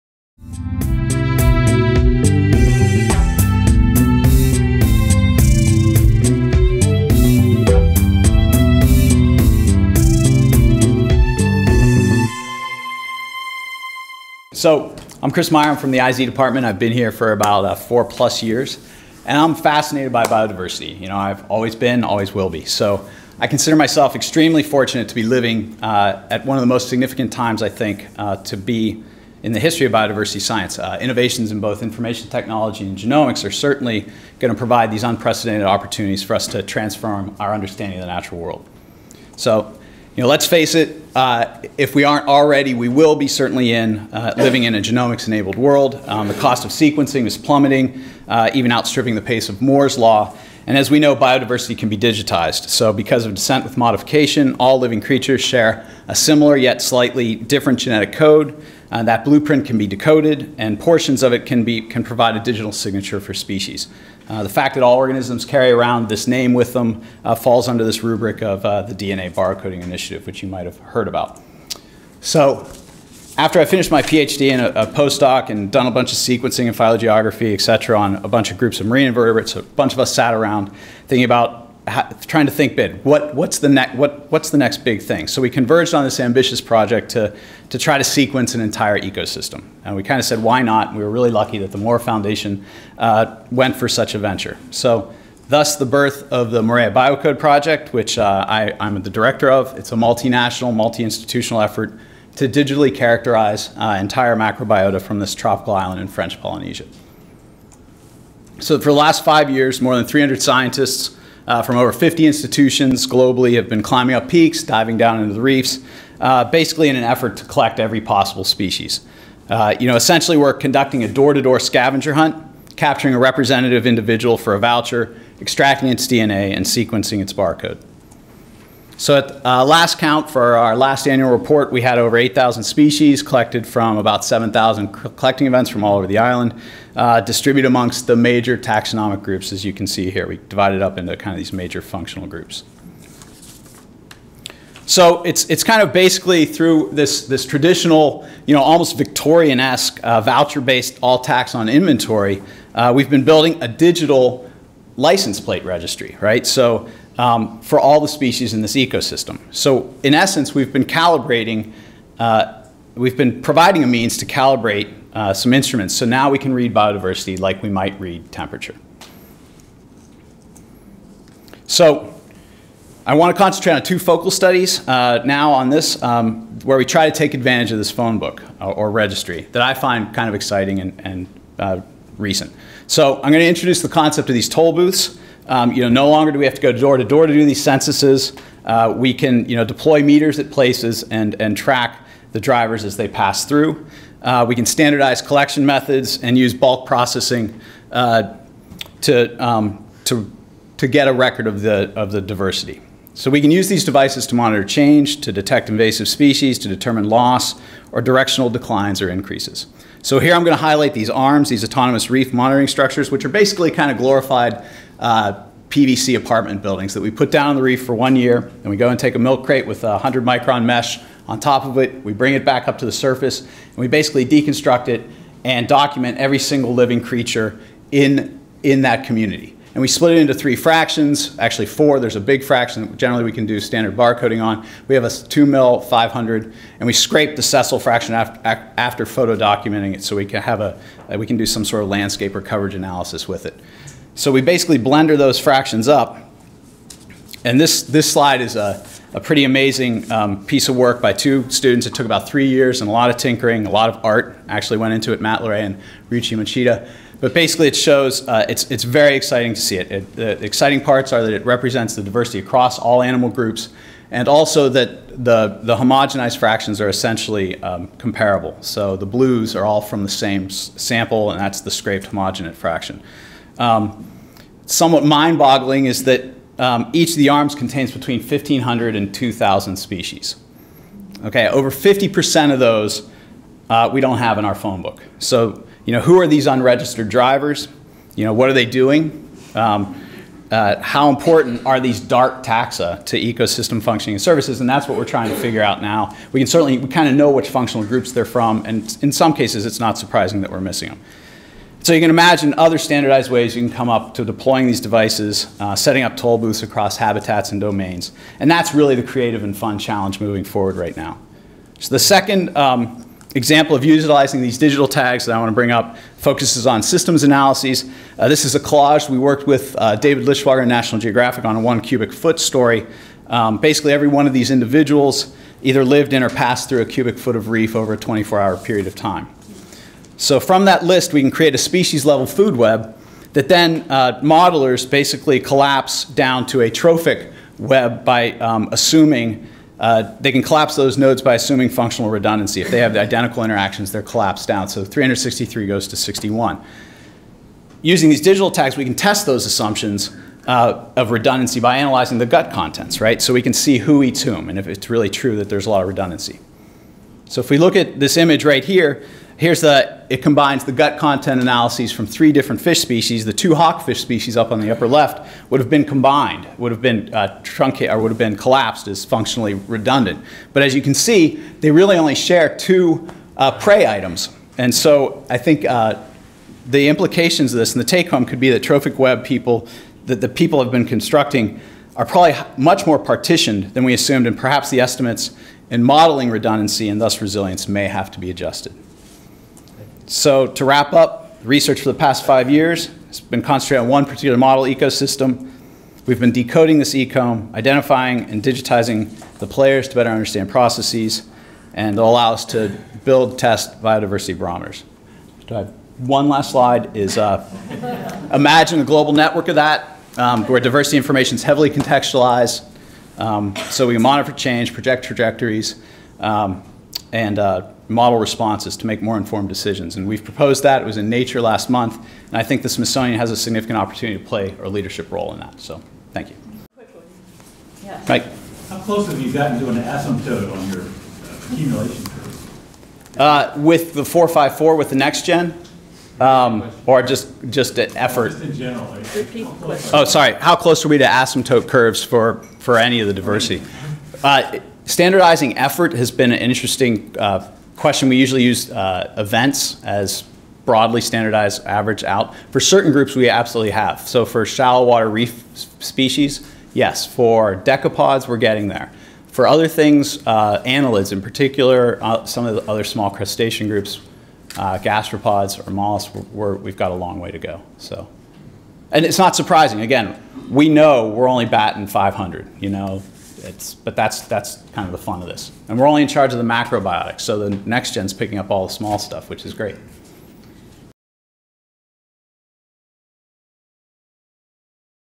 Digitizing Diversity for a Bioliterate Planet
Chris Meyer discusses digitizing diversity for a bioliterate planet